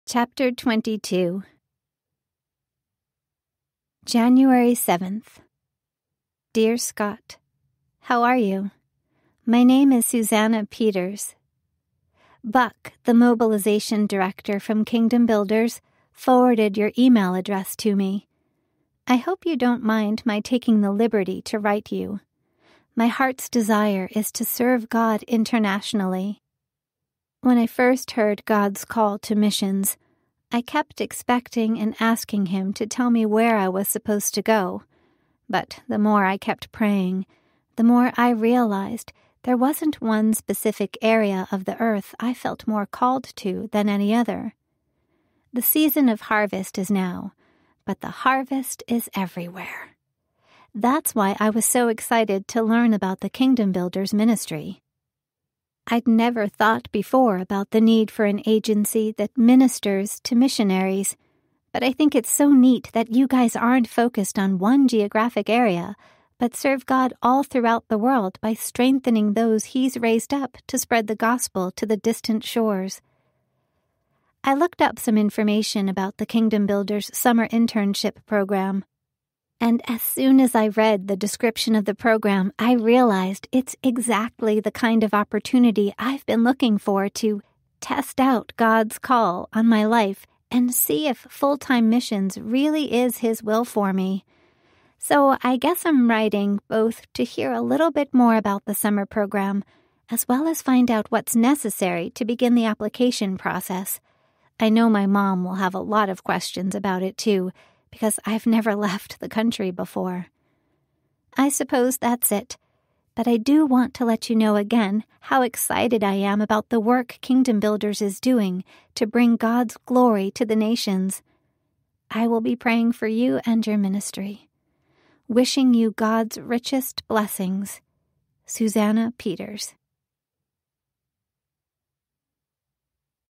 Chapter 22 What Dreams May Come a Christian romance by Alana Terry
From the queen of Christian romance comes the complete and unabridged audiobook of "What Dreams May Come," a heart-warming true-to-life novel based off the author's own unique happily-ever-after.

Listen to more Christian fiction audiobooks at the Unabridged Christian Fiction Audiobook Podcast, hosted by USA Today bestseller Alana Terry.